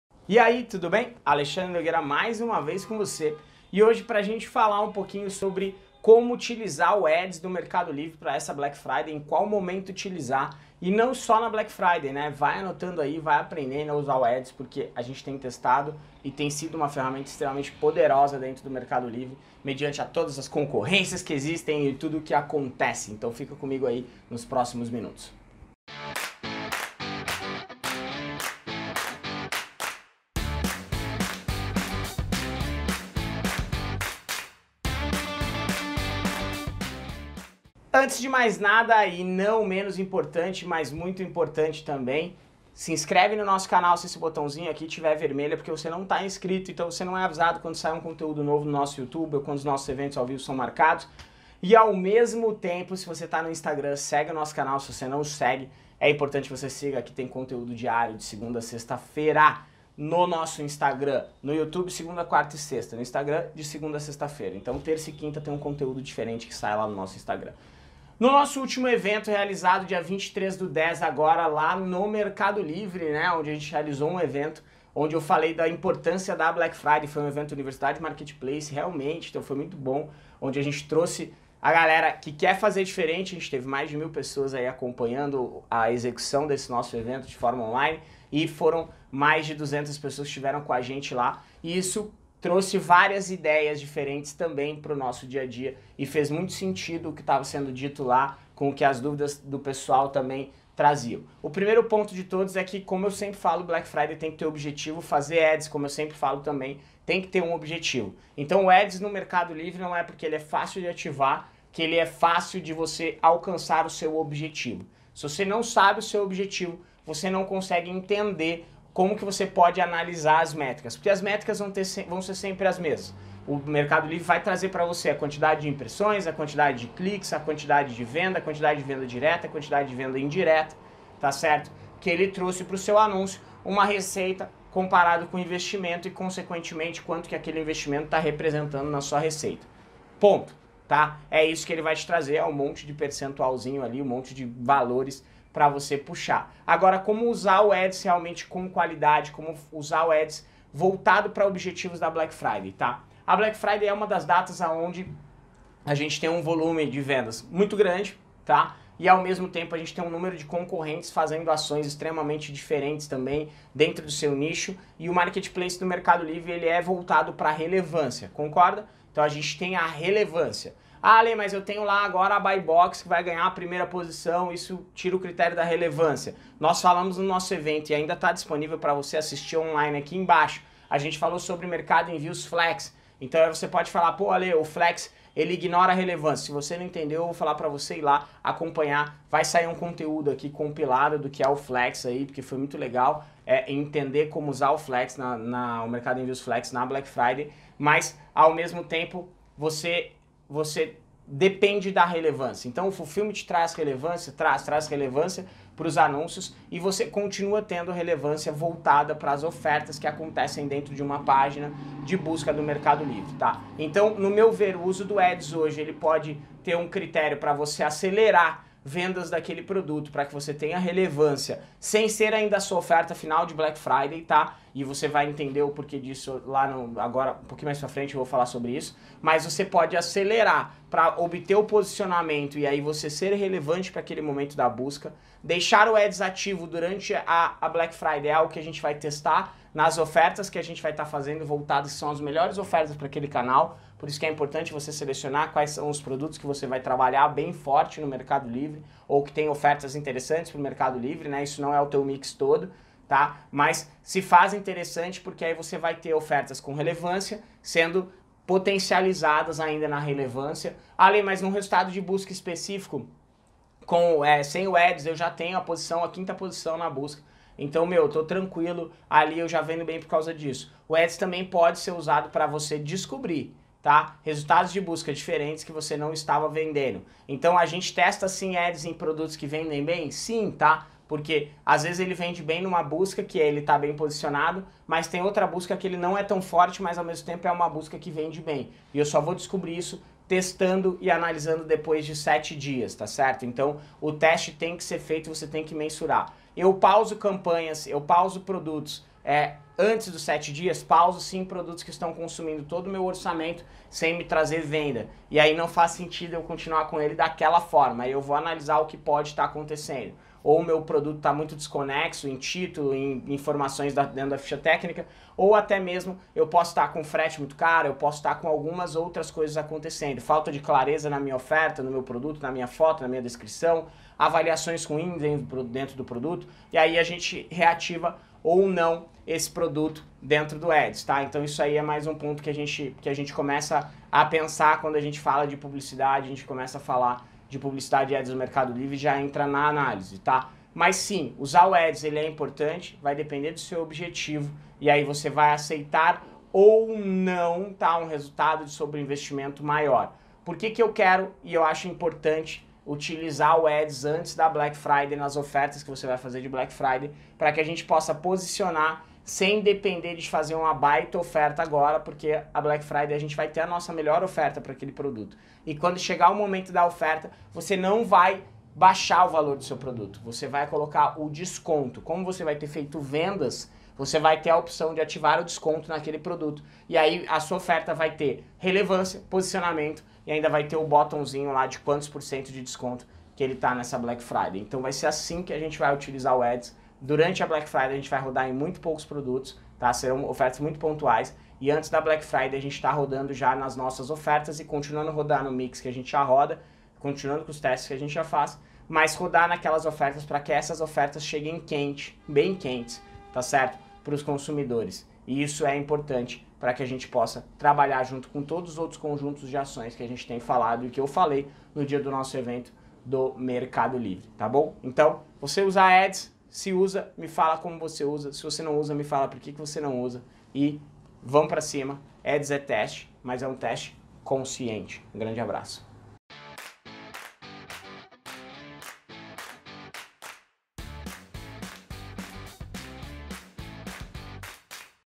Publicidade no Mercado Livre Como usar para a Black Friday PADS
📢  A ferramenta de Ads do Mercado Livre tem sido interessante não apenas no momento da Black Friday, mas também em todos os momentos, em razão do aumento da concorrência e de tantas mudanças que acontecem.
Assim como a Black Friday precisa de um objetivo, fazer Ads também precisa ter um objetivo. O fato do Ads do Mercado Livre ser fácil de ativar, não significa que seja fácil o seller alcançar seu objetivo. Sem saber o objetivo, o vendedor não terá capacidade de analisar as métricas que o Mercado Livre vai disponibilizar.
O Mercado Livre vai trazer a quantidade de impressões, quantidade de clicks, quantidade de venda direta e indireta, que ele trouxe para o anúncio uma receita comparada com o investimento e, consequentemente, quanto aquele investimento representa em sua receita.
Como usar o Ads com qualidade, voltado para objetivos da Black Friday?
Na Black Friday existe um grande volume de vendas, com os concorrentes fazendo ações extremamente diferentes dentro de cada nicho e o Marketplace do Mercado Livre é voltado para relevância.
No evento que fizemos dia 23/10/2019 na Melicidade, que você ainda consegue assistir no link acima, falamos de Mercado Envios Flex e compensa assistir porque foi explicado como ele pode ser utilizado na Black Friday.
O fulfillment também traz relevância, além do seller continuar a ter relevância para as ofertas que acontecem dentro de uma página de busca dentro do Mercado Livre.
O uso do Ads pode ter um critério para o vendedor acelerar vendas de um determinado produto, para ganhar relevância, sem ser ainda a oferta final de Black Friday para aquele produto.
Deixar o Ads ativo na Black Friday é uma técnica que vamos testar, mas apenas nas ofertas que serão as melhores ofertas para determinado produto no canal.
O Ads também pode ser usado para o seller descobrir resultados de busca diferentes que não estava vendendo, testando e analisando os resultados depois de sete dias.
O teste do Ads tem que ser feito e cada empresa precisa mensurar os resultados. Recomendo pausar Ads de produtos antes dos sete dias que estejam consumindo o orçamento sem trazer venda e analiso o que está acontecendo dentro deste produto.
Ressalto que o importante de utilizar o Ads antes das ofertas da Black Friday é posicionar o anúncio antes de fazer a melhor oferta. Com isso, no momento de colocar a melhor oferta, o vendedor não vai baixar o preço do produto e sim ativar o desconto, porque já vai ter vendas para possibilitar a ativação do desconto.
Com essa técnica, o produto terá relevância, posicionamento e ainda o desconto ativado.
Durante a Black recomendo que utilizem o Ads em poucos produtos, com ofertas pontuais.
Ads é teste, mas é um teste que deve ser feito de forma consciente.

Vamos pra cima!!!